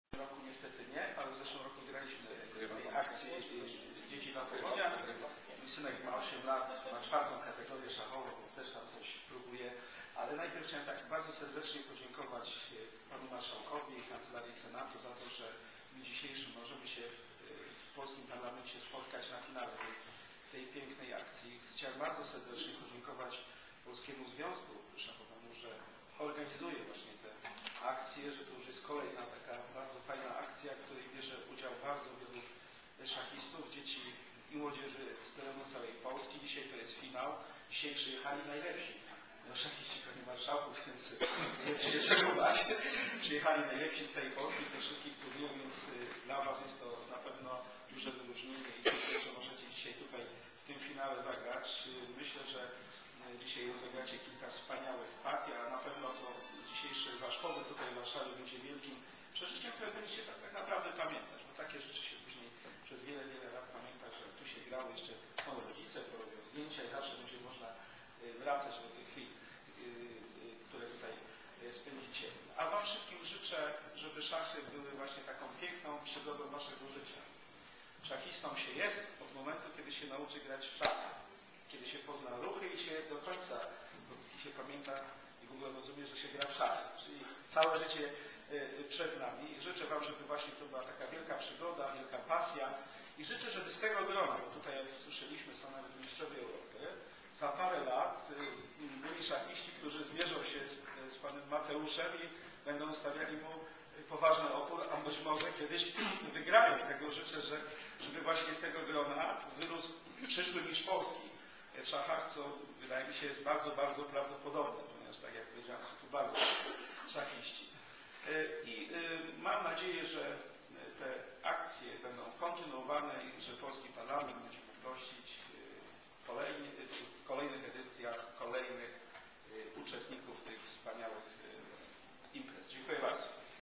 Andrzej Dera w Senacie do szachistów
Szachiści grają dla Polonii